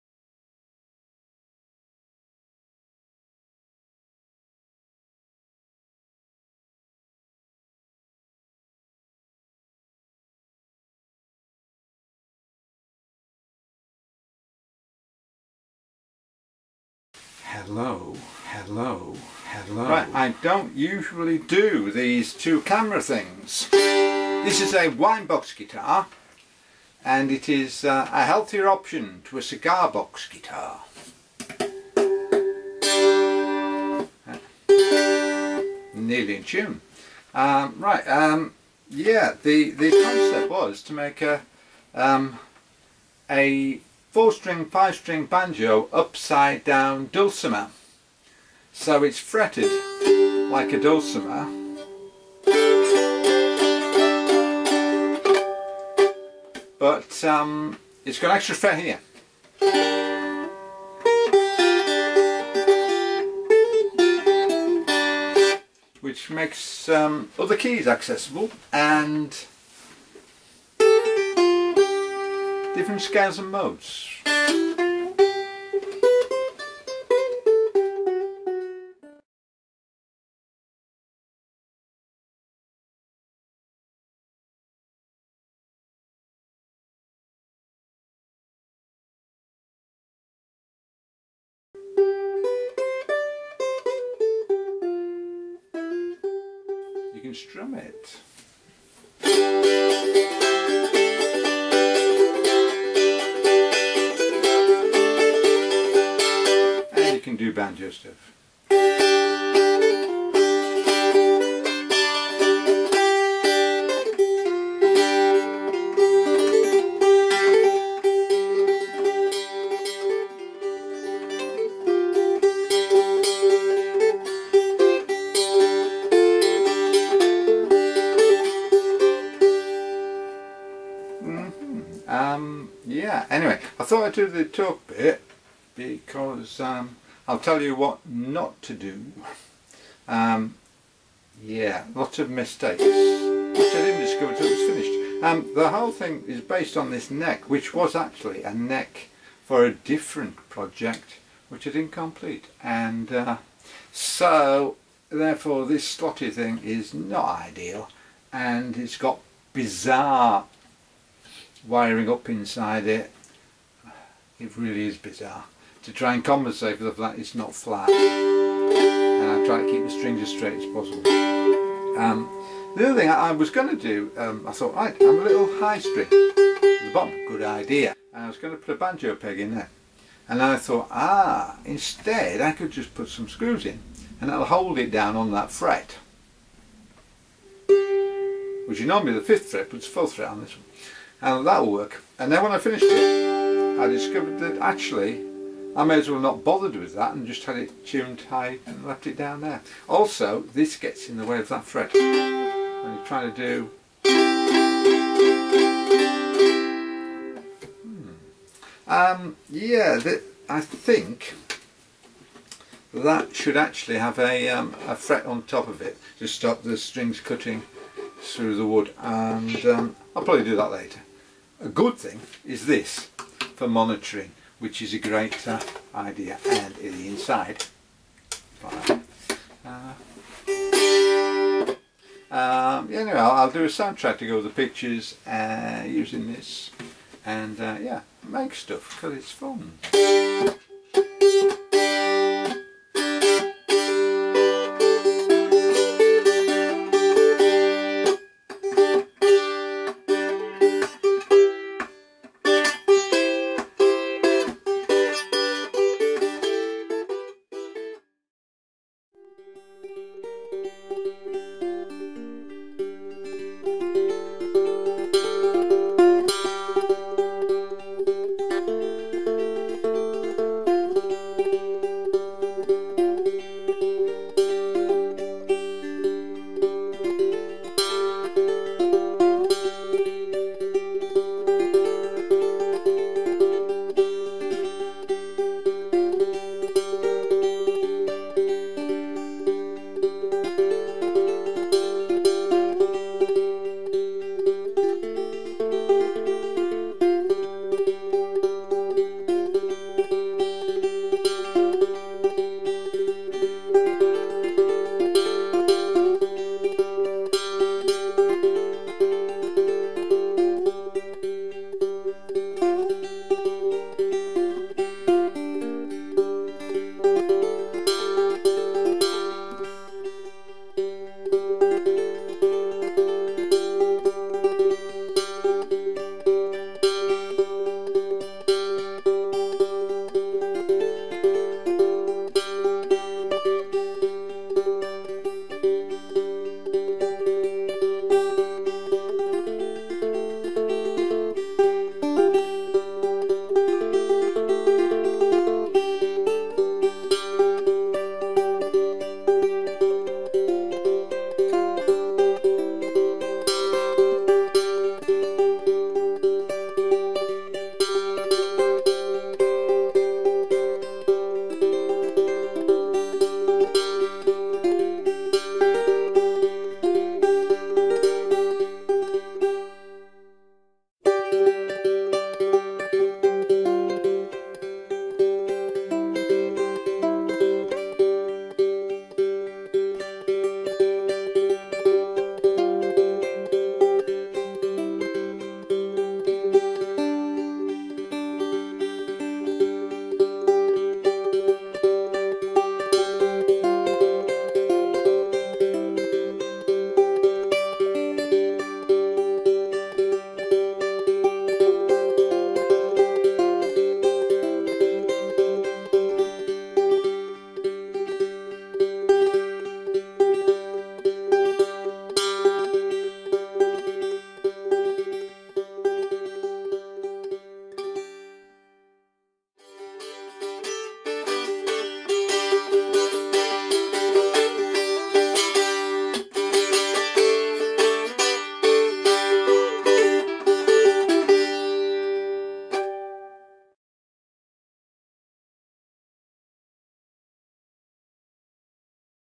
cigar box guitar - wine box 4 string 5 sting banjo upside down dulcimer actually !
Wine box 4 string 5 sting banjo upside down dulcimer
Not a high quality crafts-person build - but good fun

I usually add some sort of pick up but, in this case, just left it pure

I think I may replace the lower G string (3rd string) with a heavier wound one (to curtail a lot of the twang) and raise the bridge ever so slightly (also need to finalise the intonation)